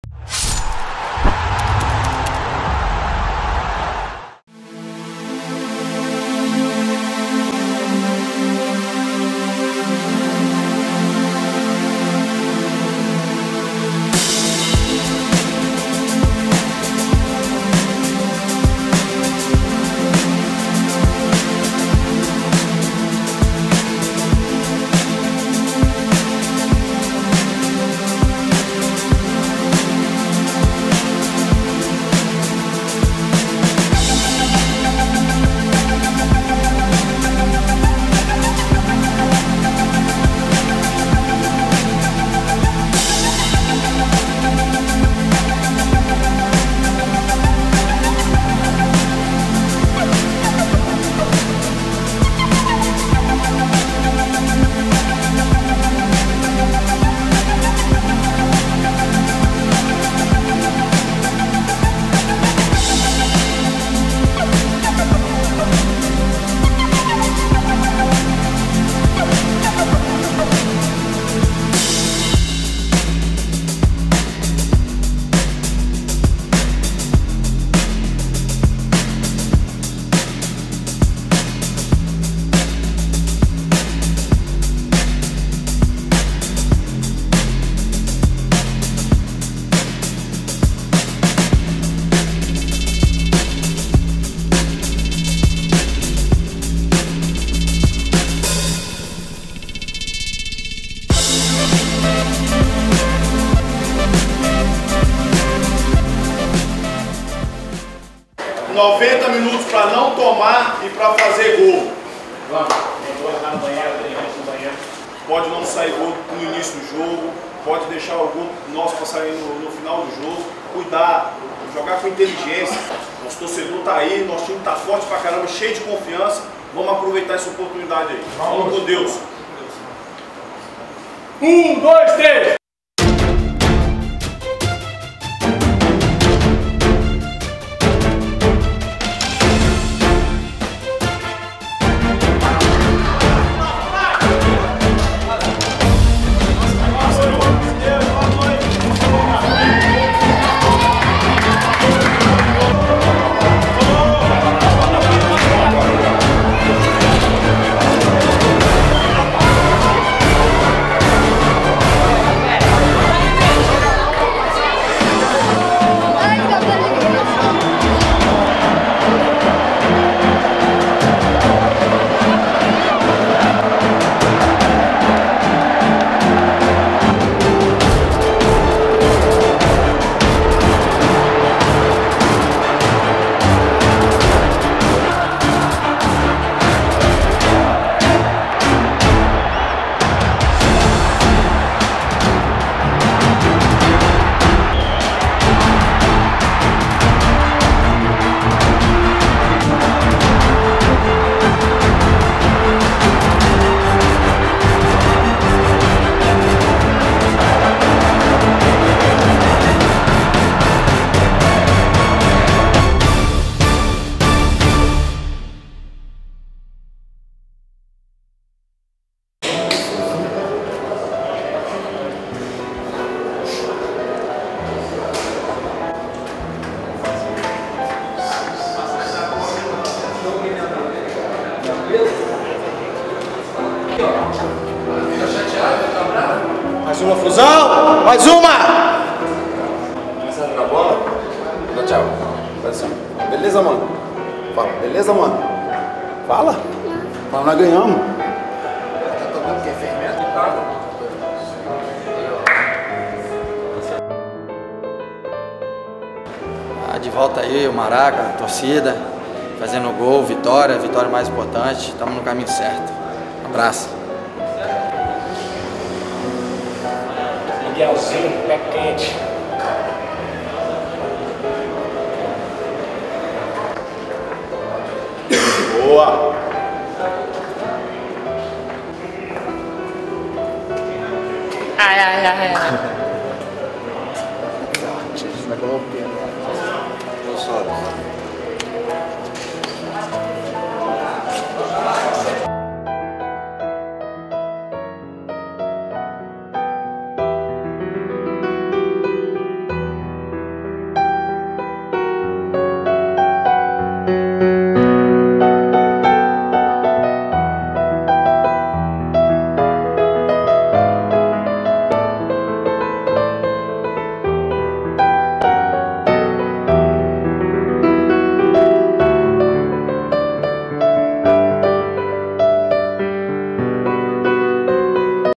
FluTV - Bastidores - Fluminense 2 x 0 Goiás - Campeonato Brasileiro 2014
Mais de 40 mil torcedores presenciaram a excelente atuação do Fluminense, no Maracanã, em dia marcado por homenagens ao Casal 20. Em campo, os jogadores se empenharam e deram continuidade à festa para os eternos ídolos Assis e Washington. Confira os bastidores de mais uma vitória do Fluminense no Campeonato Brasileiro de 2014. O Tricolor derrotou por 2 a 0 o Goiás, dia 3 de agosto. No vídeo, Fred, Conca, Henrique, Diego Cavalieri, Cícero, Carlinhos e Rafael Sobis mostram sua alegria após a partida.